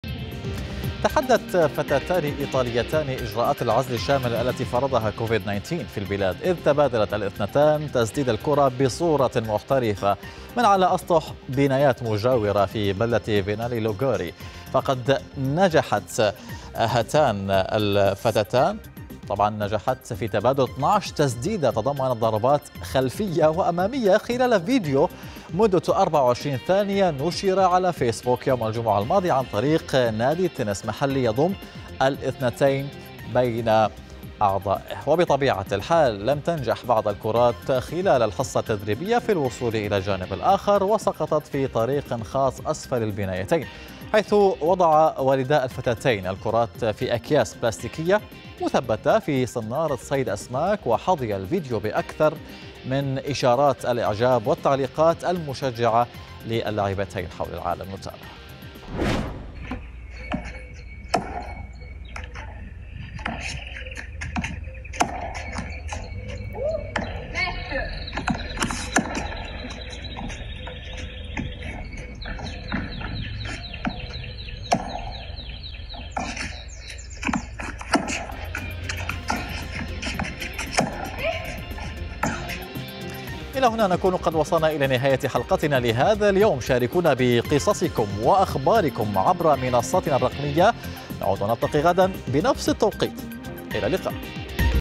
ابتكار لعبة تنس جديدة من أسطح المنازل بسبب كورونا | منصات
ابتكار لعبة تنس جديدة من أسطح المنازل بسبب كورونا